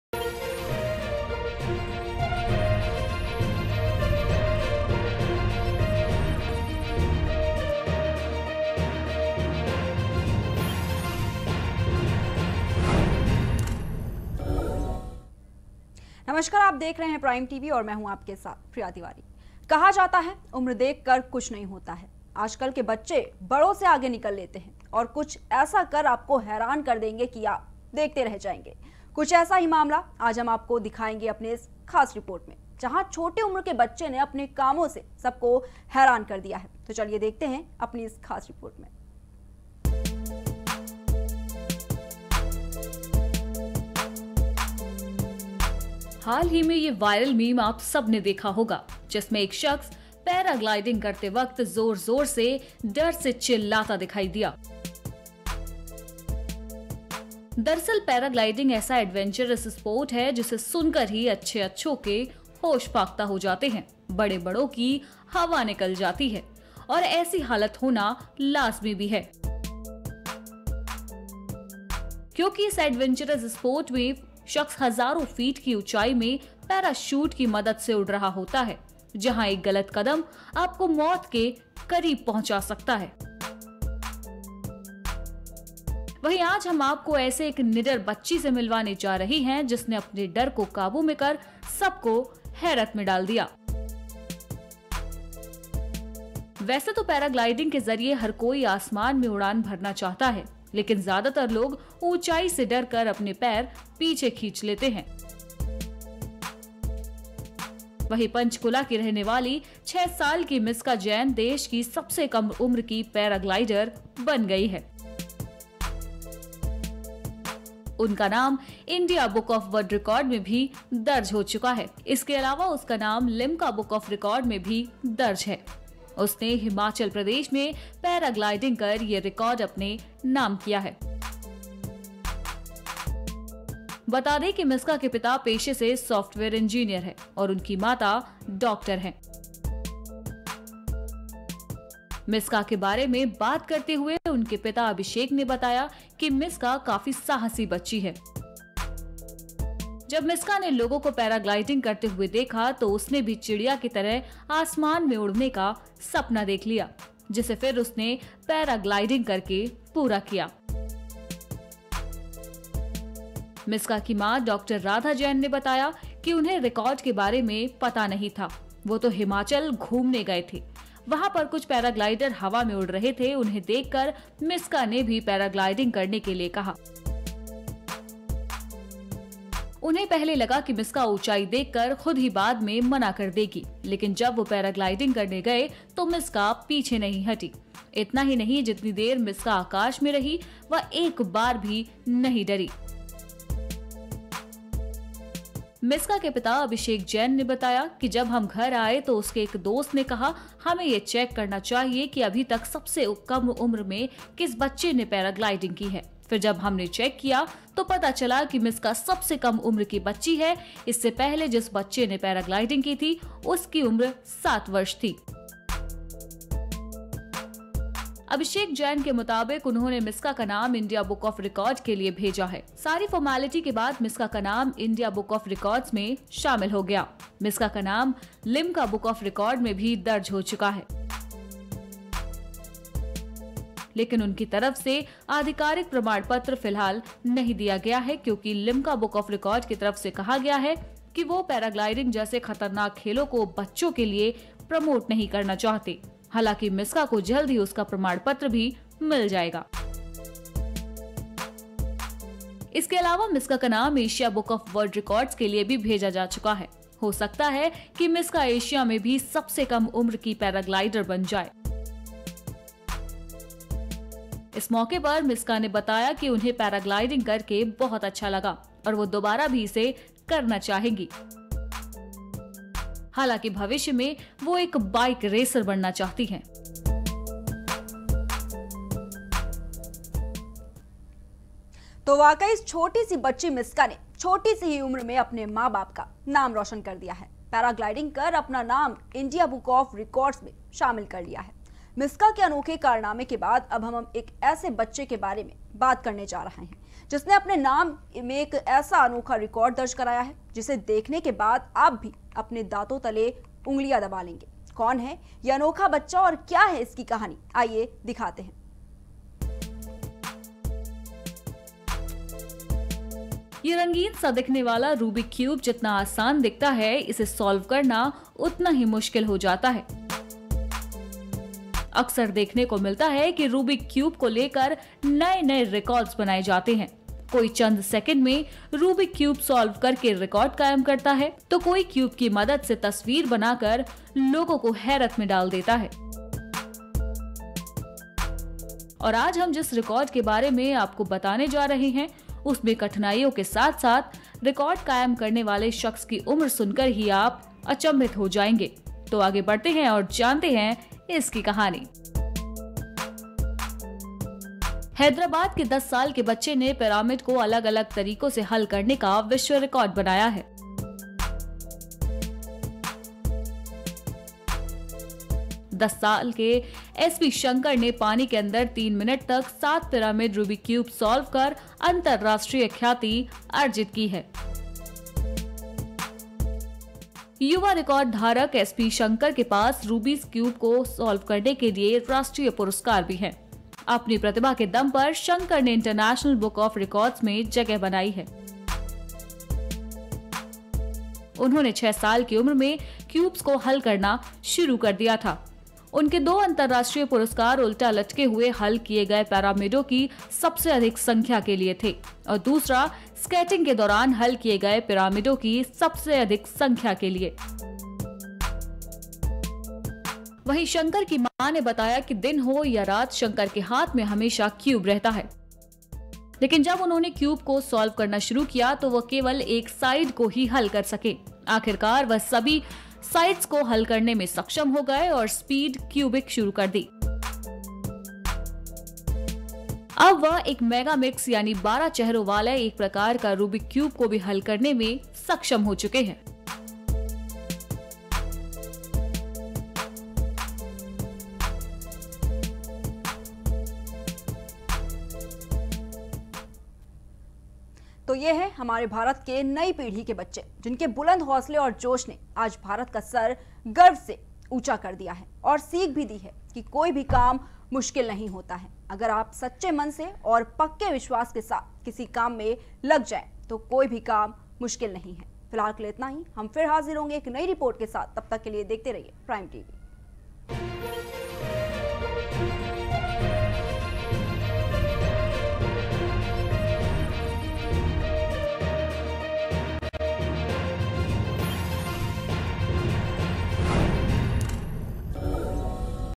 6 साल की बच्ची ने कर दिया हैरान | Exclusive || Prime Tv
6 साल की बच्ची ने कर दिया हैरान | Exclusive || Prime Tv

3 मिनट में पानी के अंदर सॉल्व किये 7 क्यूब

बच्ची ने किया नाम रौशन